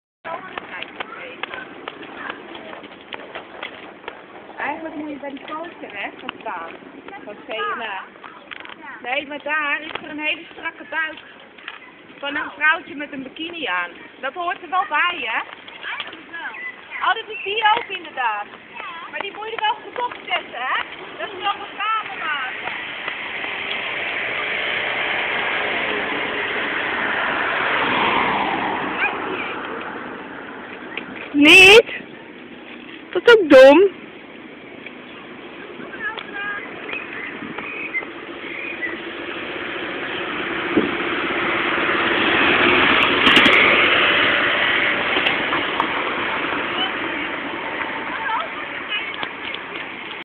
Public Fitness in Holland
okay, we totally lost our mind when we filmed this. we were 'fitnessing' at the busstop. don't know why.. we were just plain crazy :p as usual. :)